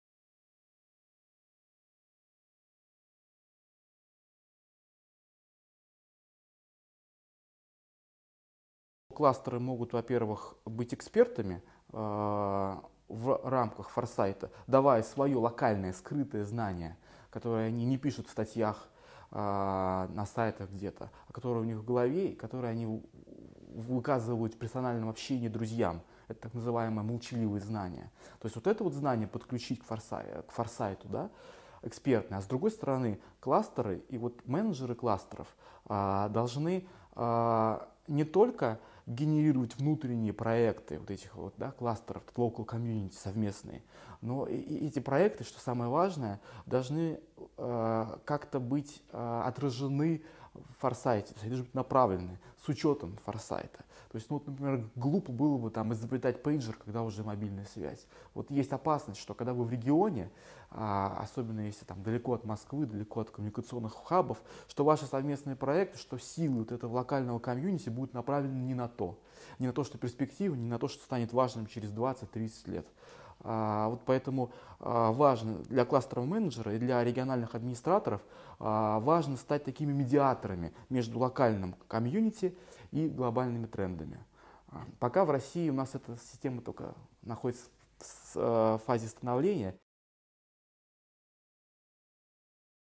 Евгений Куценко зачем кластерам форсайт
Инструменты форсайта кластерные менеджеры могут использовать двояко. С одной стороны, выступая экспертами в рамках проектов по долгосрочному прогнозированию, с другой — развивая с опорой на их результаты местные сообщества. Об этом говорил на IV Международной научной конференции «Форсайт и научно-техническая и инновационная политика» заведующий отделом кластерной политики Института статистических исследований и экономики знаний НИУ ВШЭ Евгений Куценко.